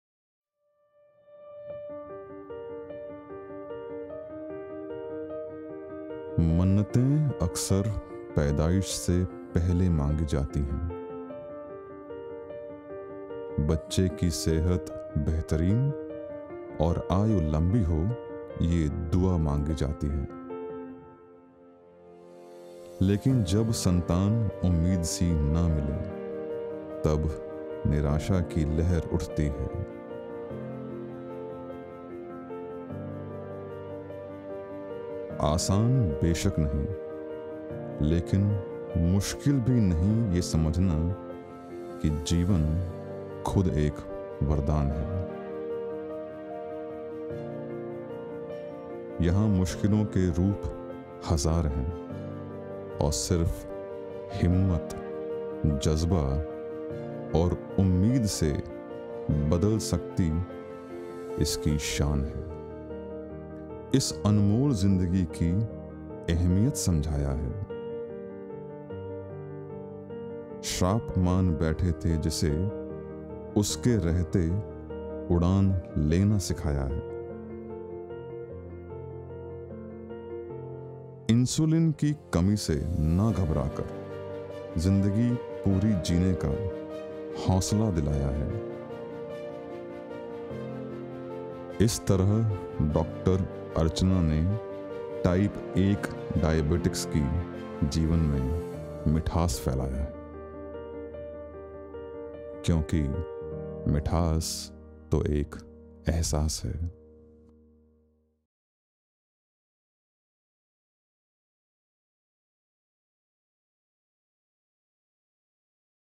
Udaan - Opening Wings of your Heart | a Poetic Narrative by Dr. Archana Sarda | Meethas Ek Ehsaas
Udaan was founded by Dr. Archana Sarda in early 2000 with the adoption of a single child who couldn't afford insulin she needed to stay alive. Today, Udaan is a family of nearly 900 families of children with  Type 1 diabetes. Every member of Udaan has either quit sugar or limited their sugar consumption; yet, their life is filled with Meetha.

Video Credits:

Narrated by:
Central Idea: Dr. Ami Mitinkumar Shah
Written by: Afshin Sayyed
Narrated by: Amit Gujaral
DOP: Shubham Gupta, Kiren Jadhav, Pulkit Chowbe
Post Production: Team Set Frame Productions - Rajat Diwan, Harshraj Shindekar, Gaurav Kubal, Rashmita Mahendra Karkera, Ashish Adesh Kamble

Produced by: Zandra Healthcare Private Limited

Presented by: Metabolic Health Digest

Supported by: Torrent Pharmaceuticals Limited